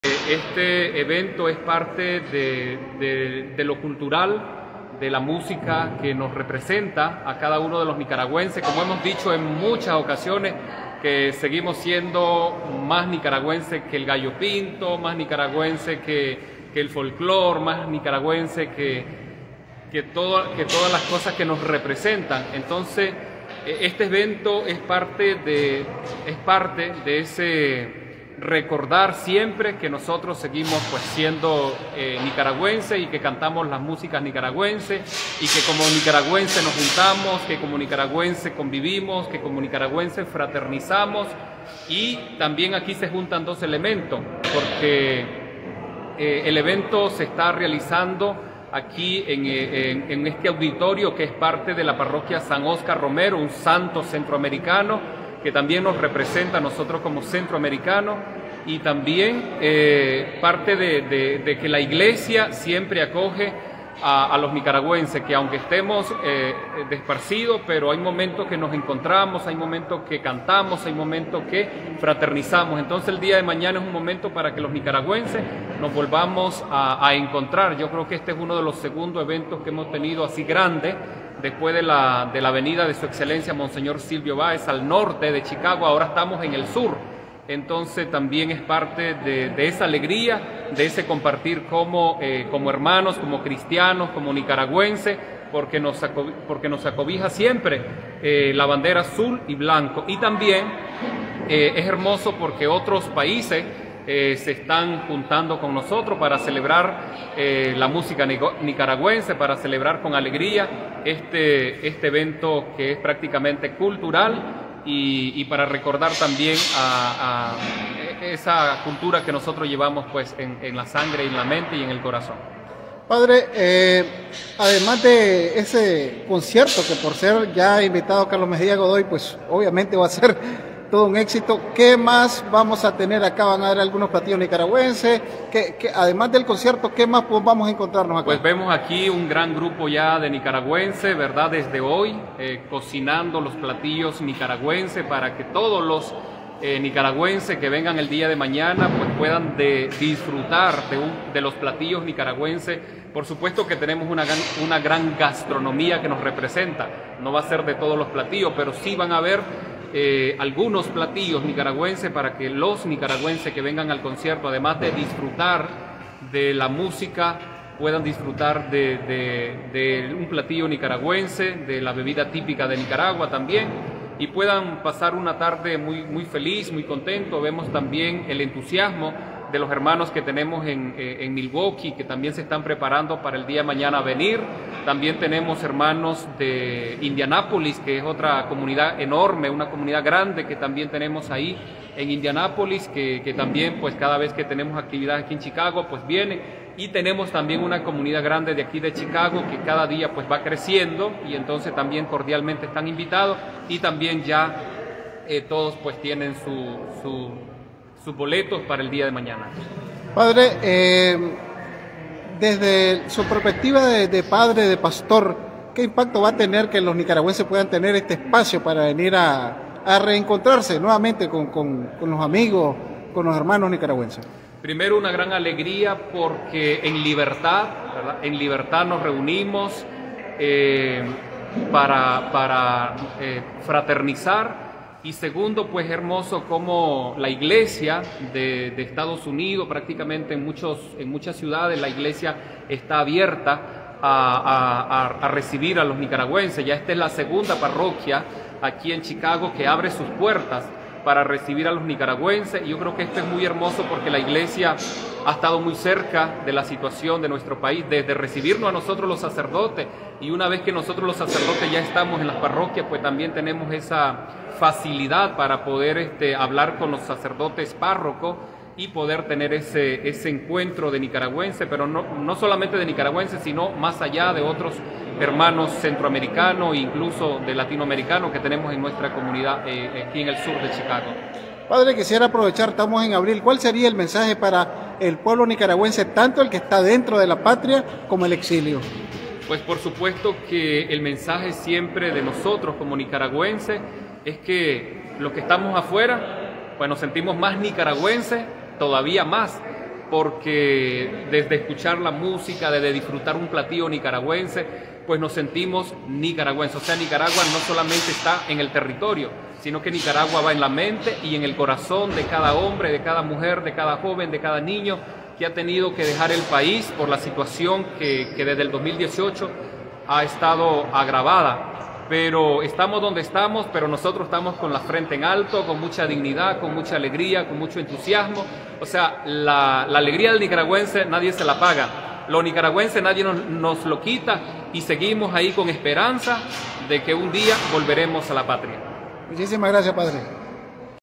Padre Erick Díaz nos habla del concierto de Carlos Mejia Godoy en Chicago.
Concierto mañana 27 de Abril en Chicago!

Continuando nuestra gira NICARAGUAMOR, con el Grupo TROVAZUL.

SÁBADO 27 de Abril a las 7.00 pm
Local: SALÓN SAN MIGUEL ARCÁNGEL, situado en 1953 OESTE con calle 48. Los esperamos!